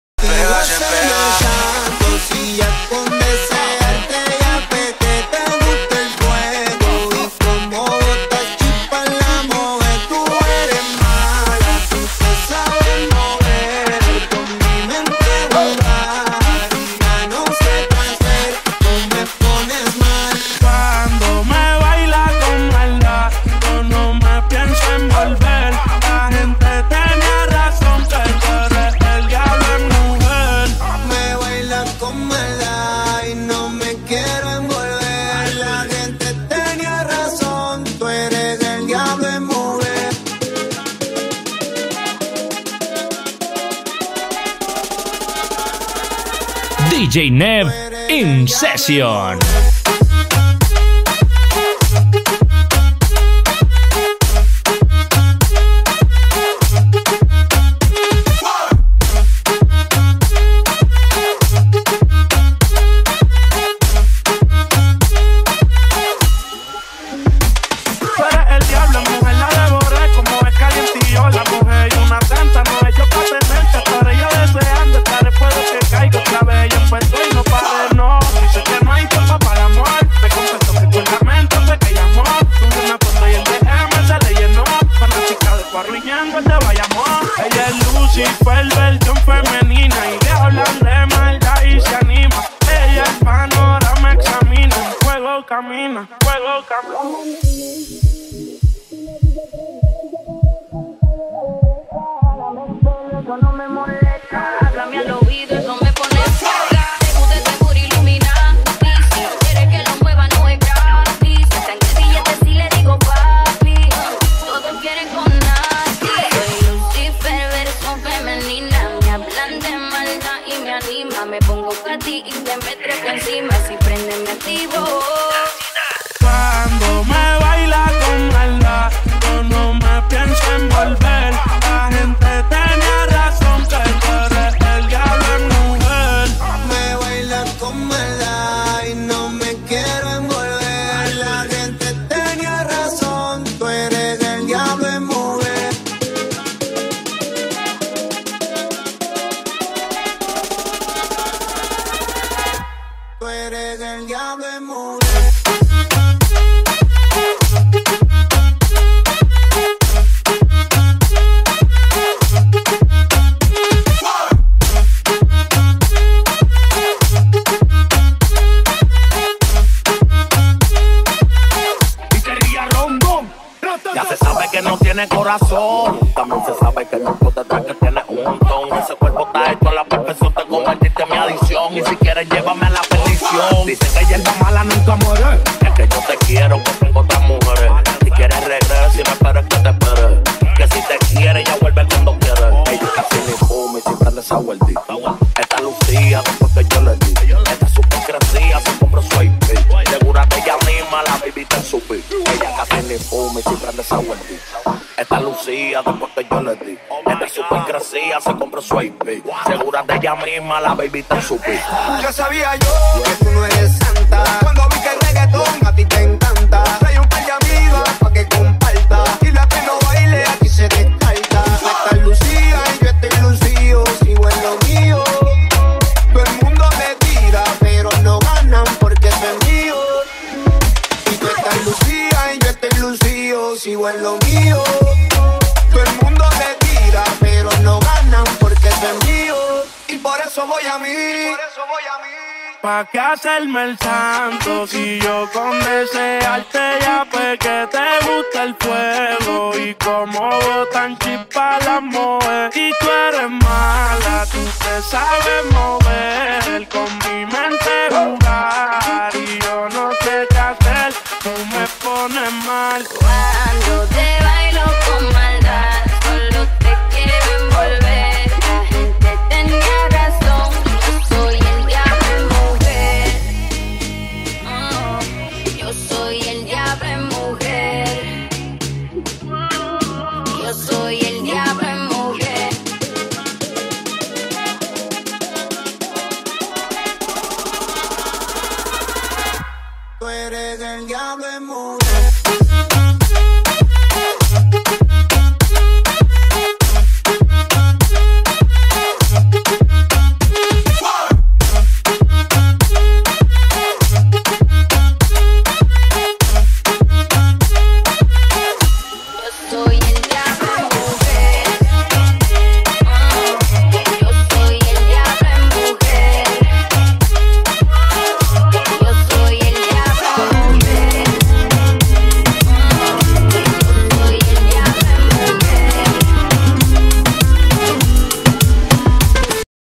13. Dj Nev Sesion Noviembre 2020 (Reggaeton, Latin y Dance)
2020